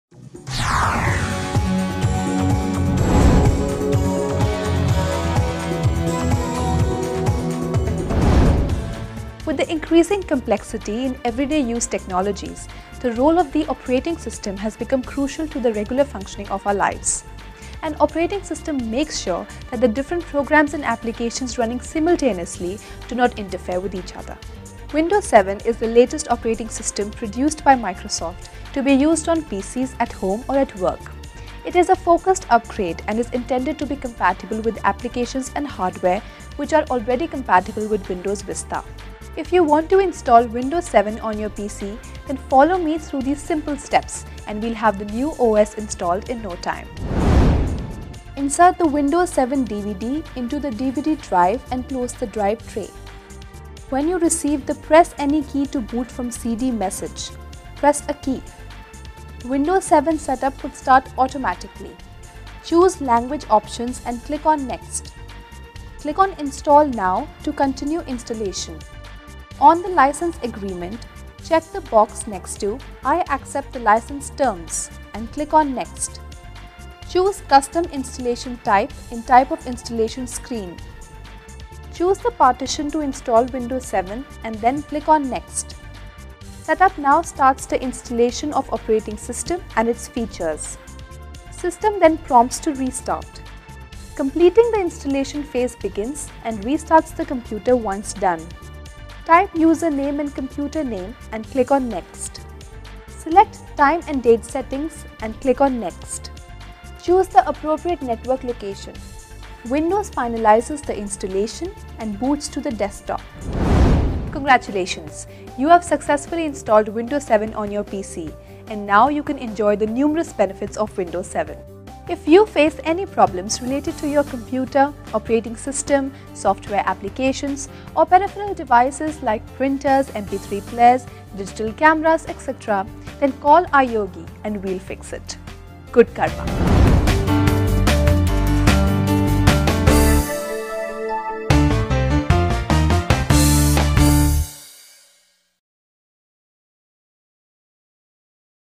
How to Install Windows® 7
Steps to perform a custom installation of Windows® 7 using its DVD.
**Check the 'video response' given below for steps to 'Format' the hard drive partition before installing Windows 7**